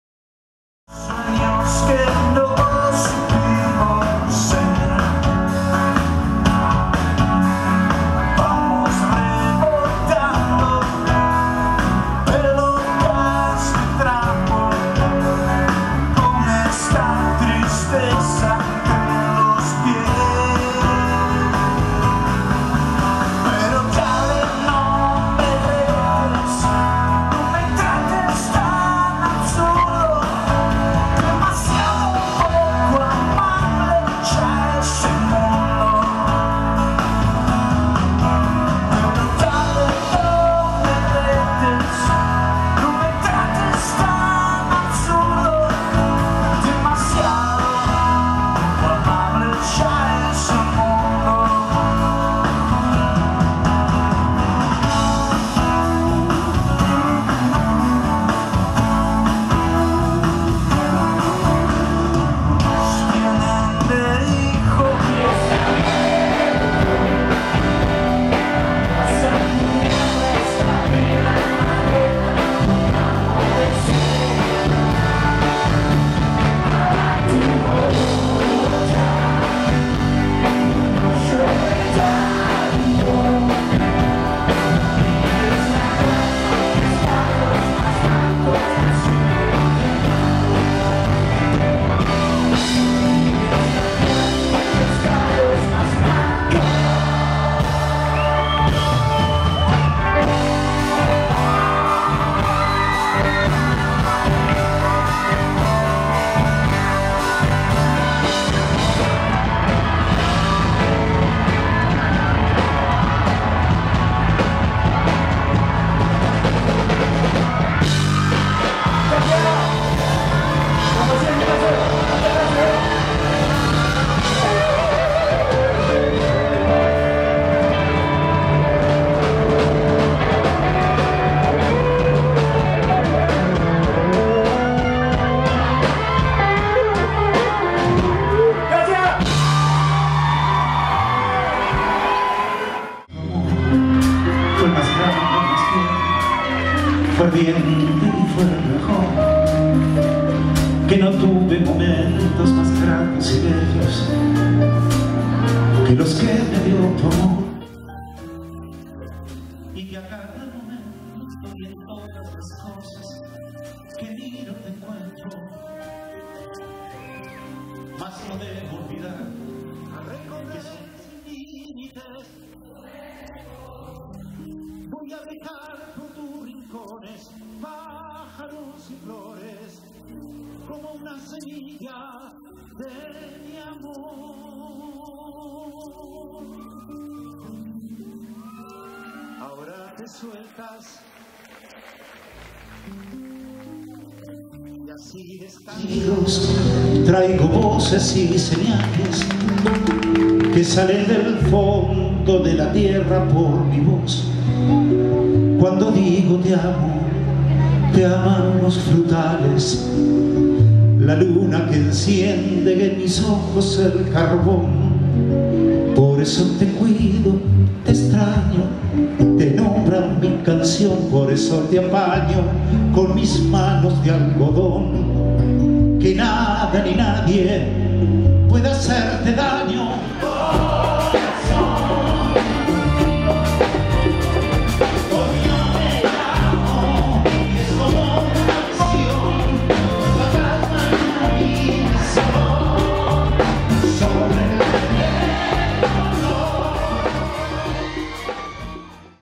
Iván Noble y Paz Martínez le cantan a la mujer en Guaymallén
Los artistas se presentaron en la explanada municipal, en el marco de los festejos del Día Internacional de la Mujer.